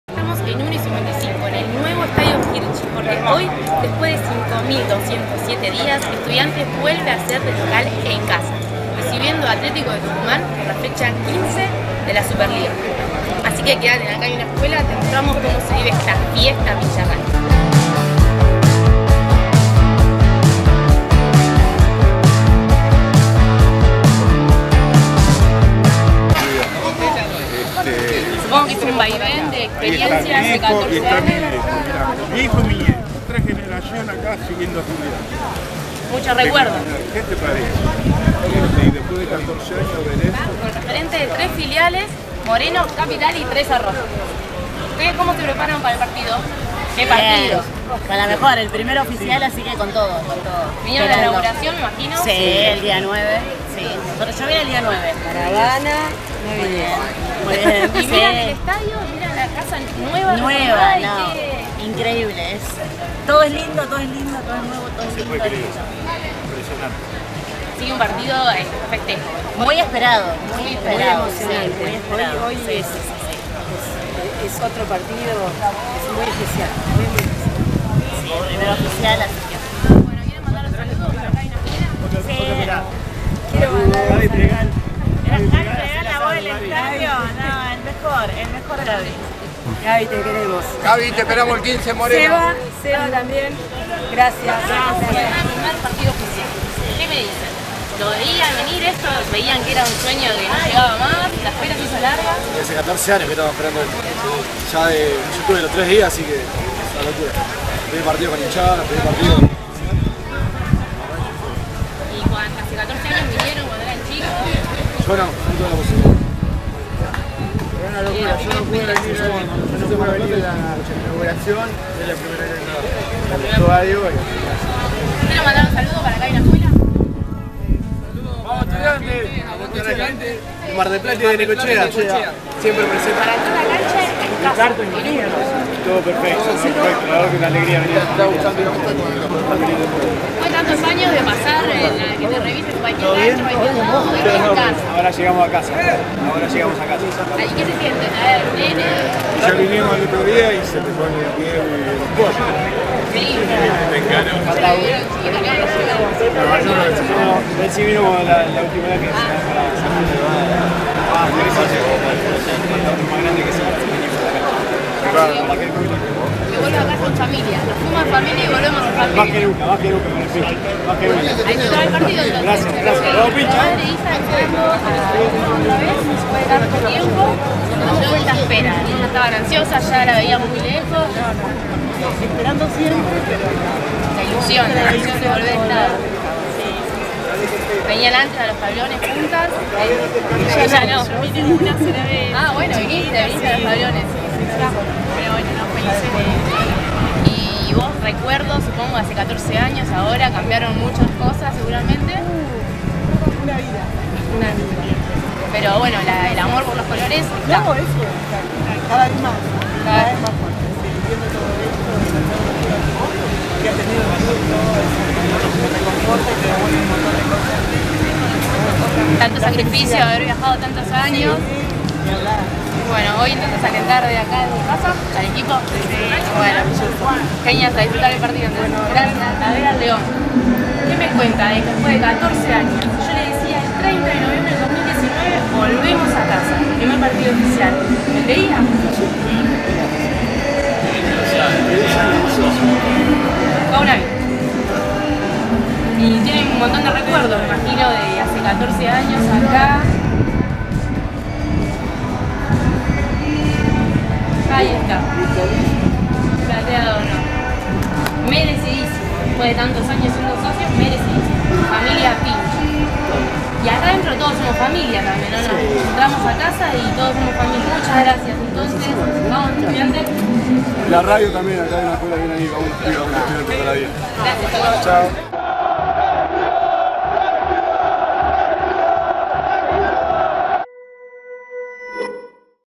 Primer partido oficial en UNO y hablamos con la gente.
Testimonios de la familia pincha, minutos antes de la gran fiesta.

Acá hay una escuela

Cámara : Emiliano Oberto

Edición : Gonzalo Oberto

Notera: Natalia

De Buena Fé Producciones